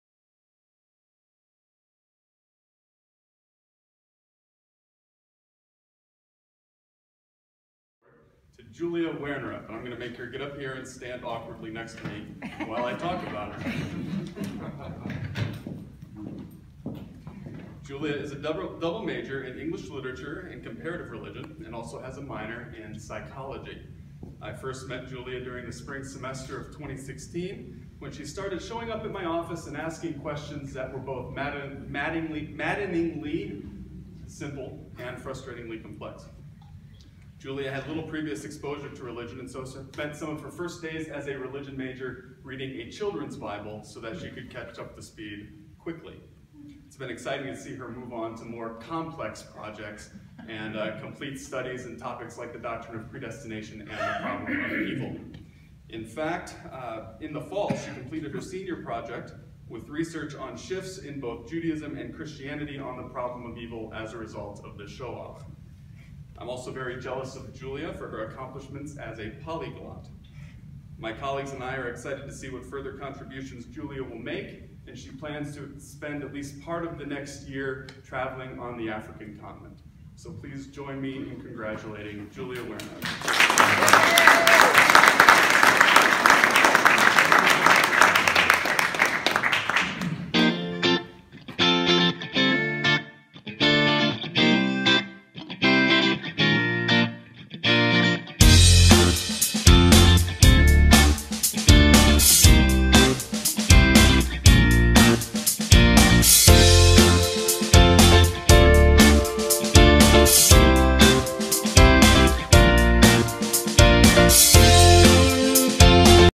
2018 Lindenwood University Harmon Religion Award Presentation
The 2018 Lindenwood University Harmon Religion Award was presented to Julia Waernerup by Dr. W. Travis McMaken (on behalf of the LU Religion faculty) on April 22, 2018 as part of the Lindenwood University Honors Convocation.

The Harmon Religion award recognizes academic excellence in the study of religion. This award is named in honor of the Rev. Dr. Lloyd B. Harmon, father of the late Lindenwood University Board Chairman Raymond Harmon and Professor Emerita Jeanne Harmon Huesemann. Dr. Harmon was a longtime chaplain and professor of religion at Lindenwood.